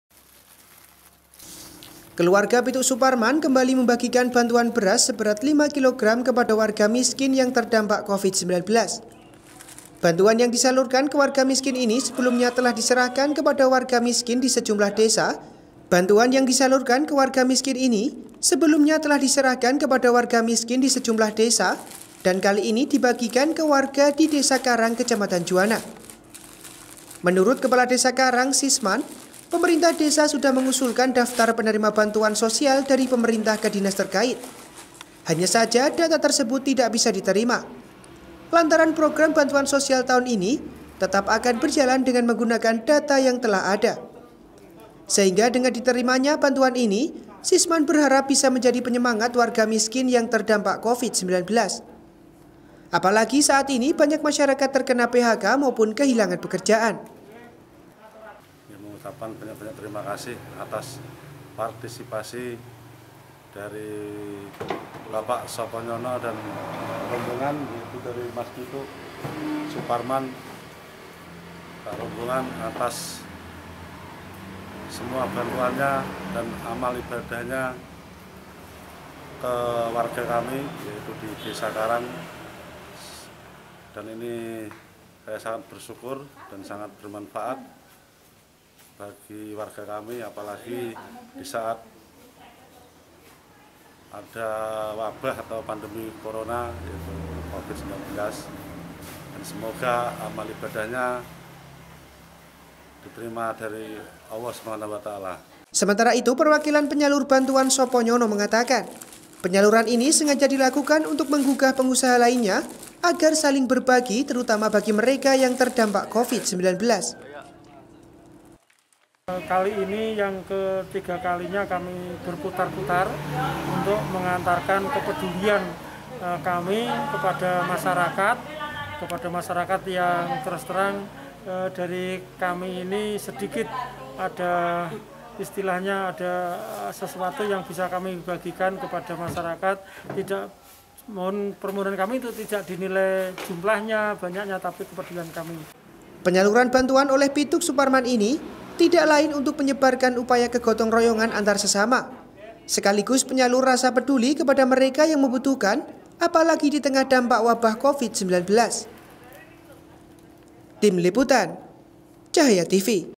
PITUK SUPARMAN HARAPKAN PENGUSAHA DI PATI TERGUGAH UNTUK BERBAGAI DI TENGAH PANDEMI
TERUS PANCING PARA PENGUSAHA DI PATI UNTUK BERBUAT KEBAIKAN/ KELUARGA PITUK SUPARMAN KEMBALI SALURKAN BANTUAN BERAS KE WILAYAH DESA KARANG KECAMATAN JUWANA